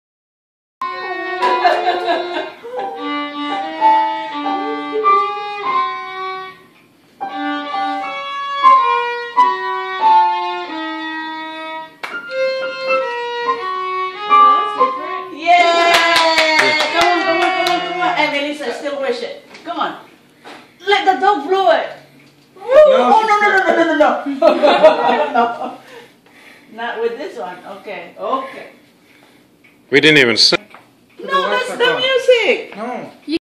Maelisa's 18th birthday
Sister Mylene on the piano. And brother Marc on the Violin. Just beautiful beautiful music! Music you will never forget once you hear It's harmonic sounds. Maelisa loving it.